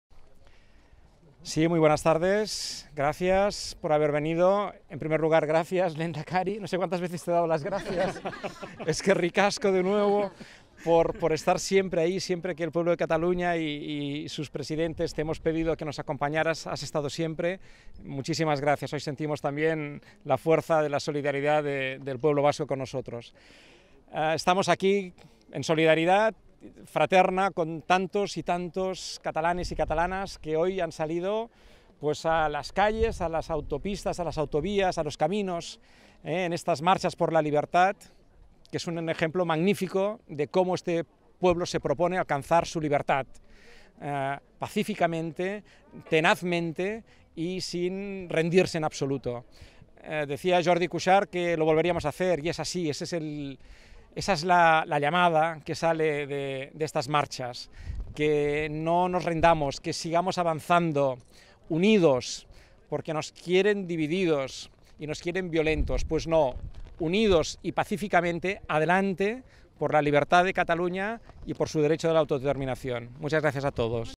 Quim Torra: "Nos quieren divididos y violentos"
El president de Cataluña, Quim Torra, ha afirmado en una manifestación que las marchas catalanas son "un ejemplo magnífico" de como Cataluña, según su valoración, se propone "alcanzar su libertad. Torra ha declarado que les quieren "divididos y violentos". Por ello, ha llamado a mantenerse "unidos" y "pacíficamente", seguir "hacia adelante por la libertad de Cataluña".